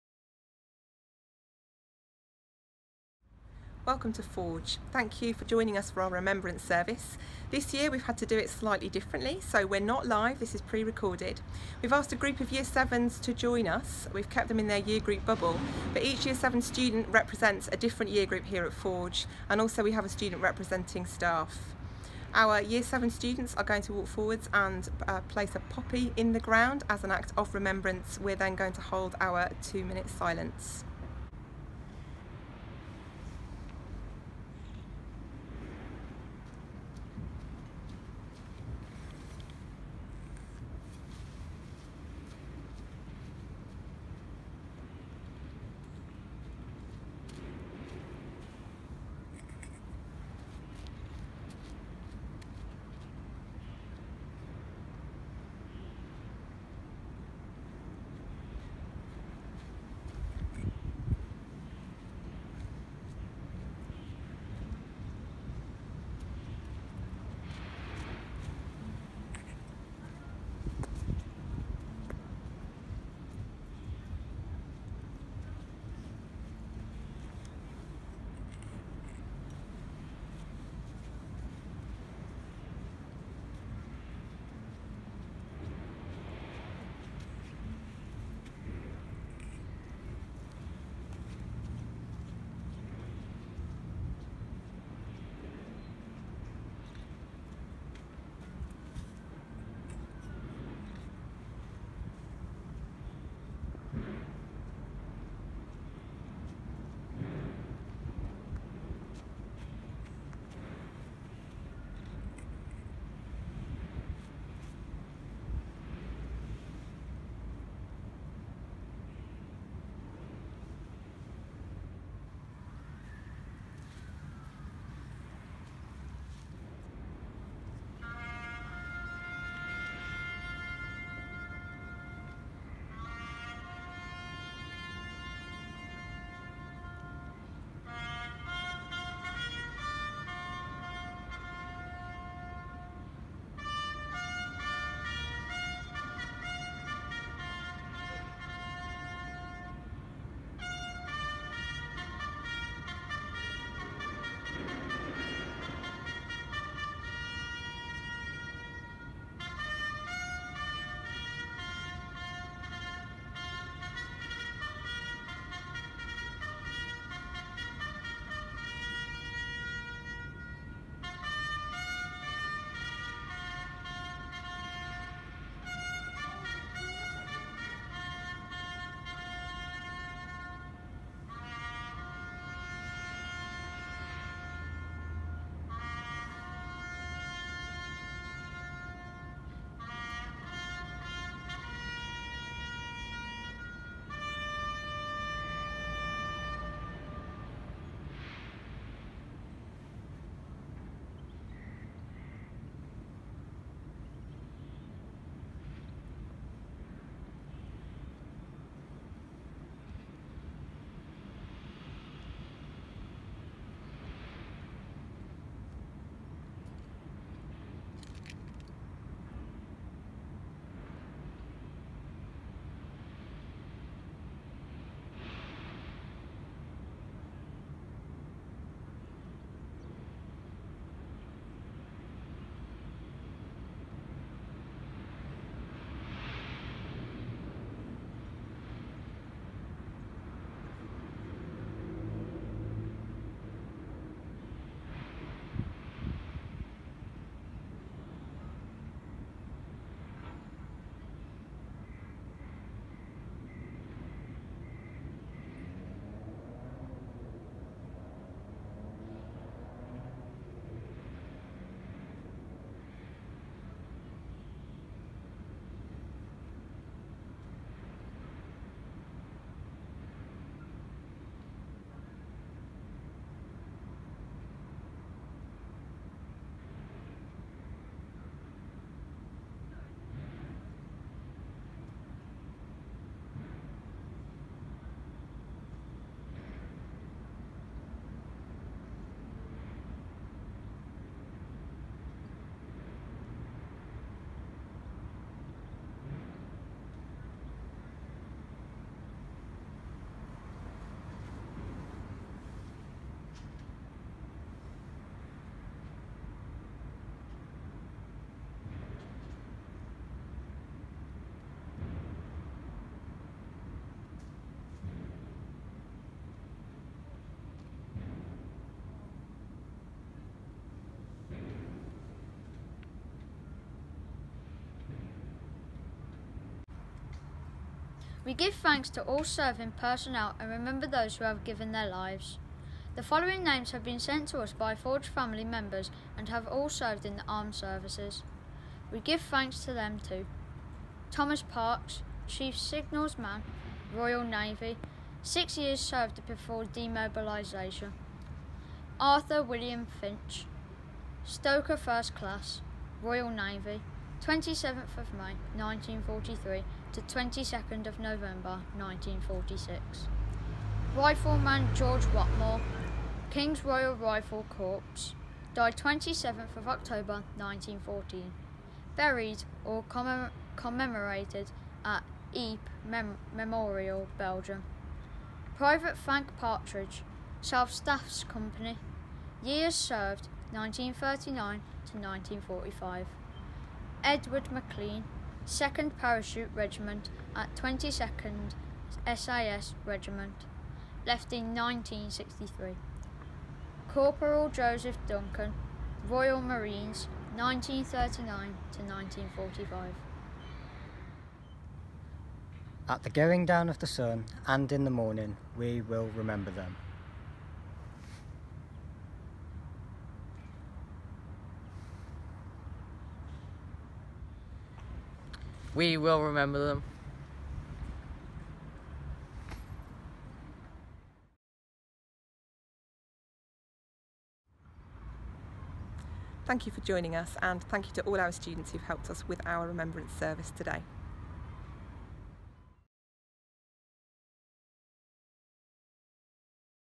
Remembrance Day with Ormiston Forge Academy
This year we have had to change the way we show our respects on RemembranceDay...

We have made homemade poppies to wear or display in our windows. Many haven't been able to attend memorials, but have reached out through letters and fundraising.

Today marks 102 years since the end of World War One. And Covid-19 may have interrupted many of our annual service, but as a nation we will never forget the men and women who fought so bravely.

Thank you to Ormiston Forge Academy for this beautiful display of remembrance on behalf of our OATfamily.